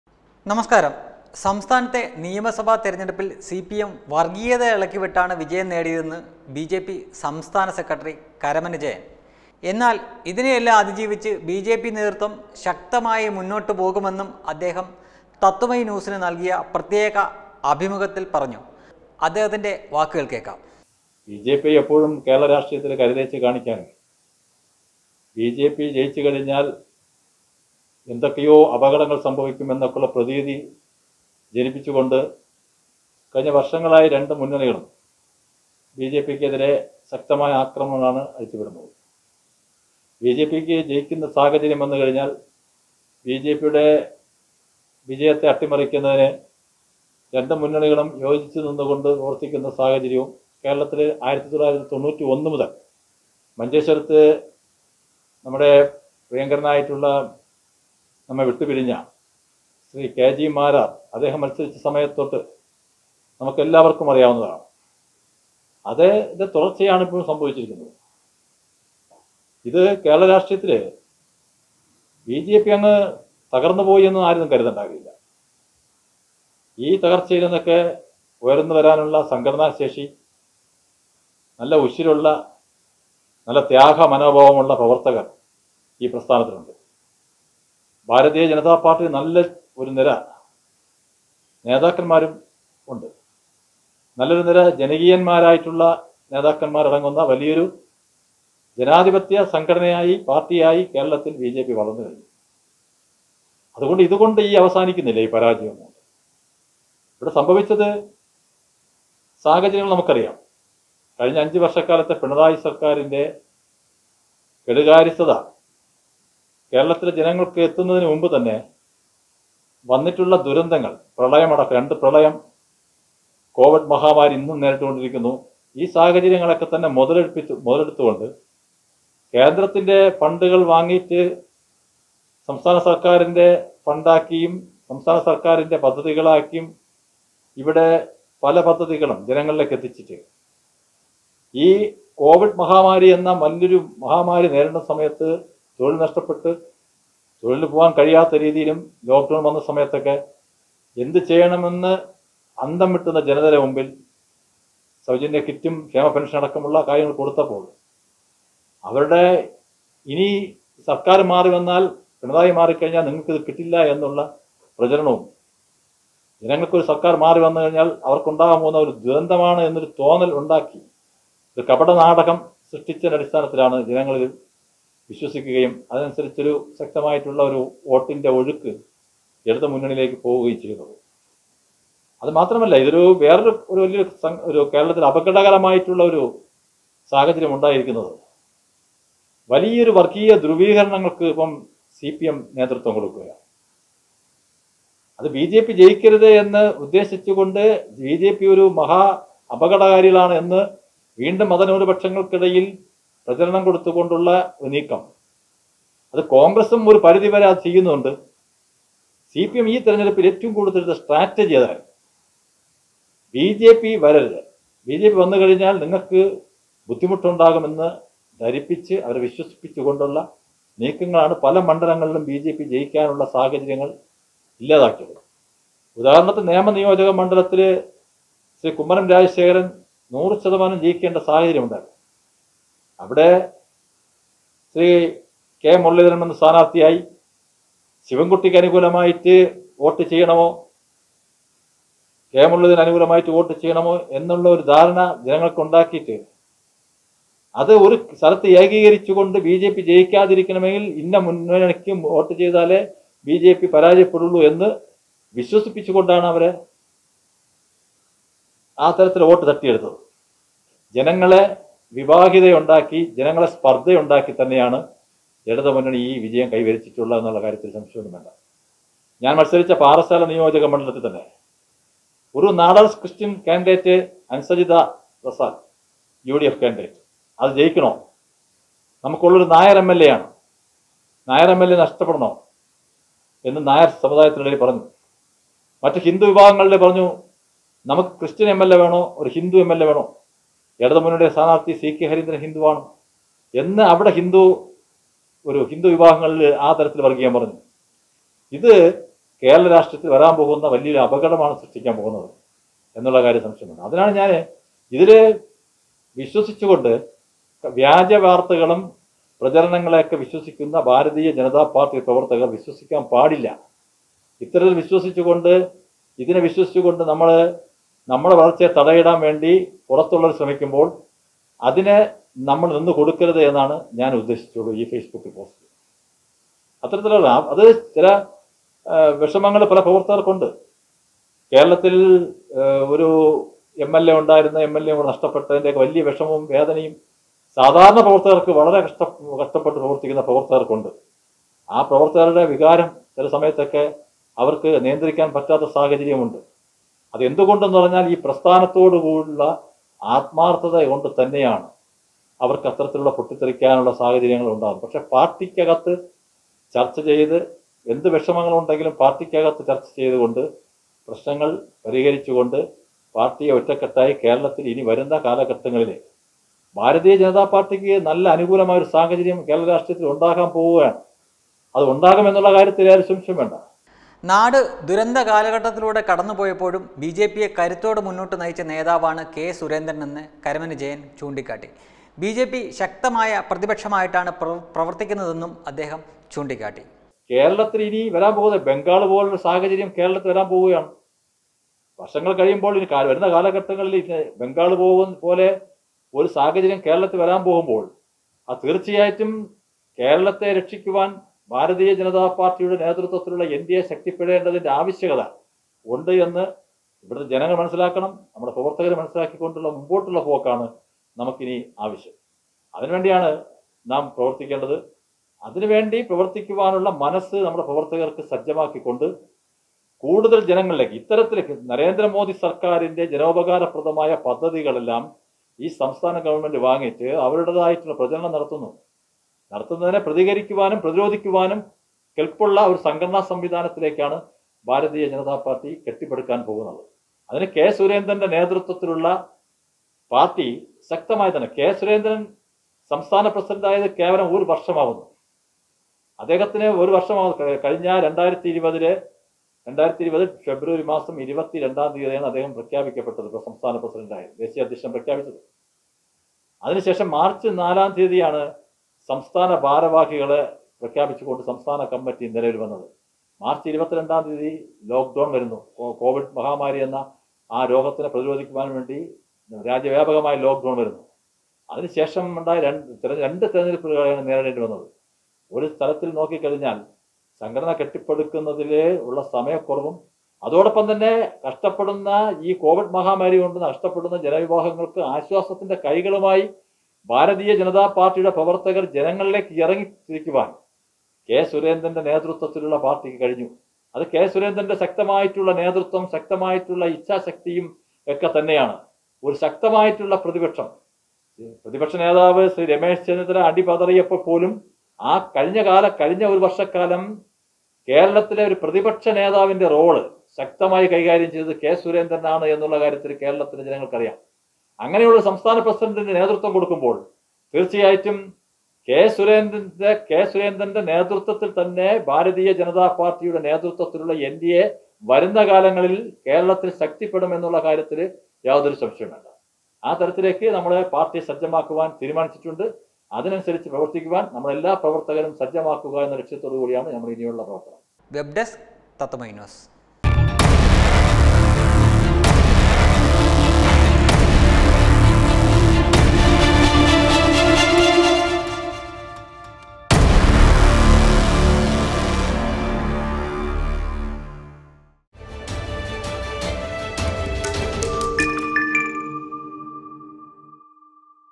സി.പി.എം ജയിച്ചത് ജാതി പറഞ്ഞ്ബി.ജെ.പി ശക്തമായി മുന്നോട്ടെന്ന് കരമന ജയന്‍
സി.പി.എം ജയിച്ചത് ജാതി പറഞ്ഞ്
ബി.ജെ.പി ശക്തമായി മുന്നോട്ടെന്ന് കരമന ജയന്‍ | KARAMANA JAYAN